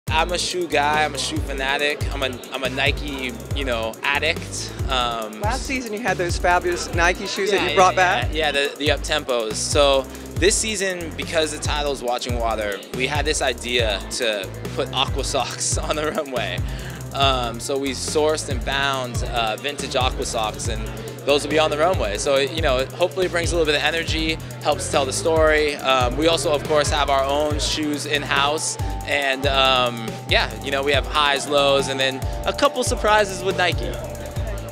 John Elliott Spring / Summer 2017 Shoe Fanatic | Global Fashion News
John Elliott Spring / Summer 2017 Behind The Scenes Exclusive Interview and Ready-To-Wear Collection by designer CREATIVEDIRECTOR.

Fashion Editor/Stylist:
Hair Stylist:
Makeup Artist:
Producer:
Casting Director:
Models: Adonis Bosso, Alexis Ordonez, Aly N'Diaye, Austin Weinreb, Daniel McSweeney, Daniel Oh, Darius Johnson, David Agbodji, Dean Zruya, Dillon Westbrock, Erin Mommsen, Fernando Cabral, Geron McKinley,

Hamid Onifade, Hunter Taylor-Martinez, Jackson Hale, Jeremy Matos, John Kolic, Jonathan Bellini, Kendall Harrison, Louis Mayhew, Max Masters, Michael Lockley, Nathaniel Dam, Noma Han, Roberto Sipos, Rockwell Harwood, Sheani Gist, Sung Jin Park, Sup Park, Wonjung Jo, Youssouf Bamba